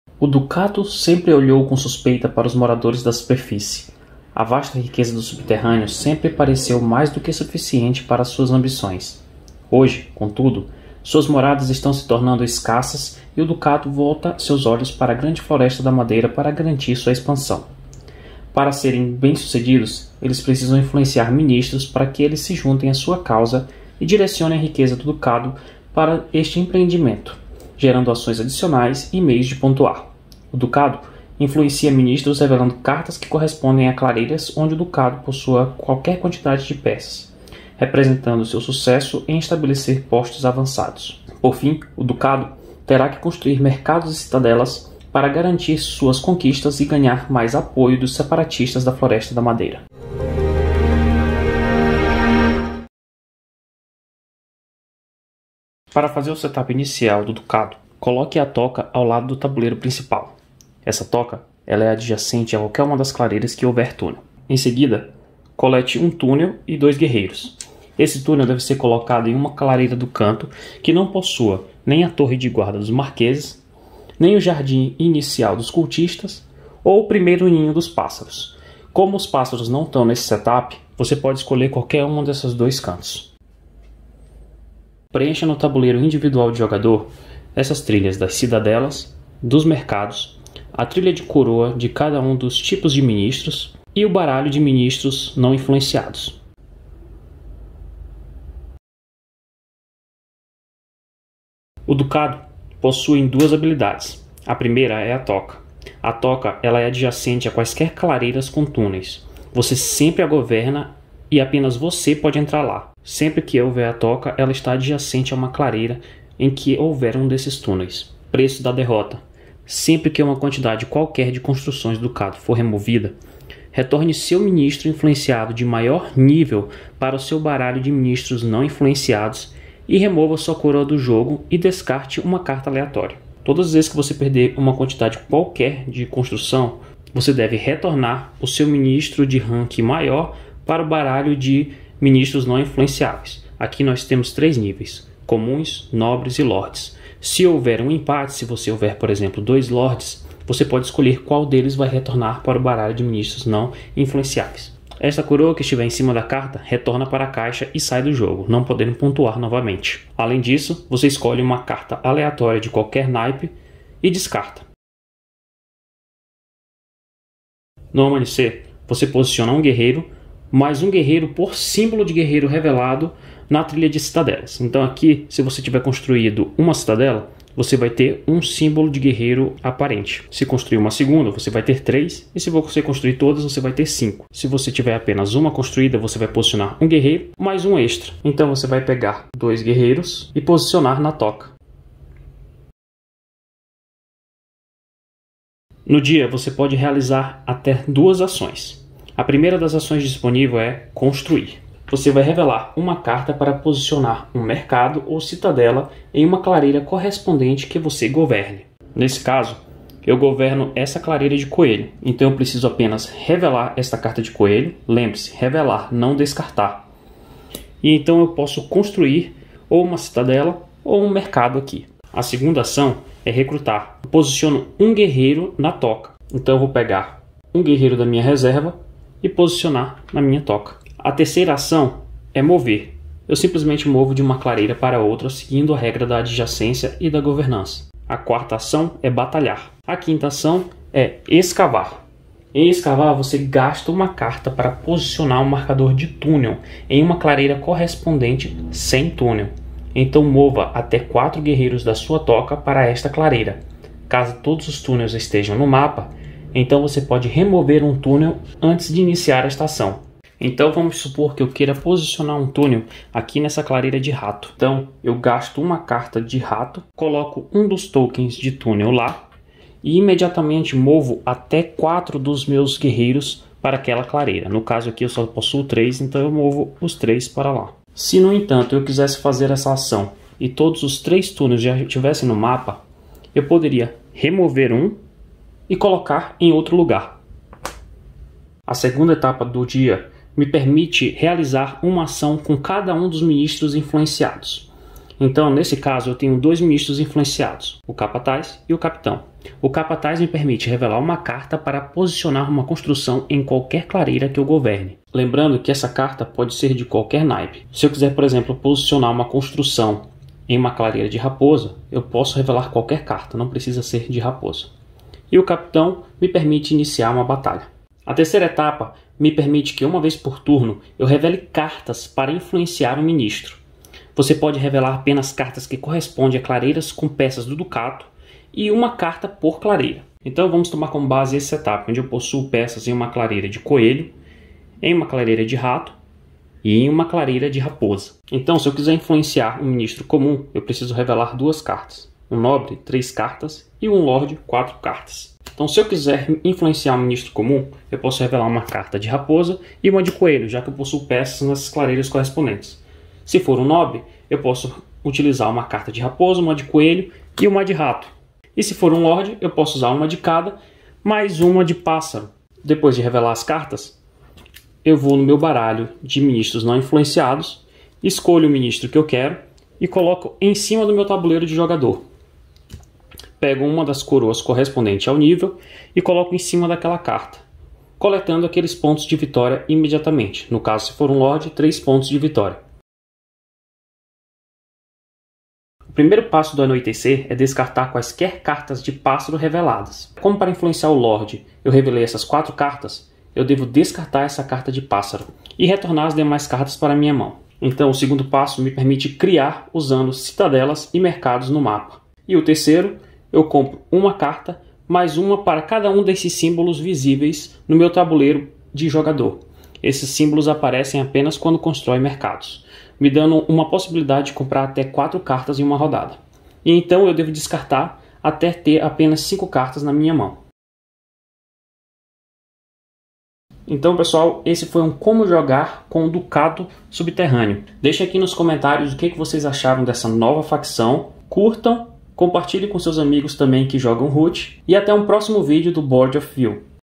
Ducado subterrâneo (Root) - Como jogar
ERRATA: NO SETUP POSICIONE TAMBÉM 2 GUERREIROS EM CADA CLAREIRA ADJACENTE, EXCETO NA TOCA.

Aprenda como Influenciar seus ministros a sair do mundo subterrâneo e tomar a floresta da madeira.

0:00 Overview
0:54 Setup
1:40 Habilidades
2:46 Amanhecer
5:14 Dia
6:24 Anoitecer
7:02 Conclusão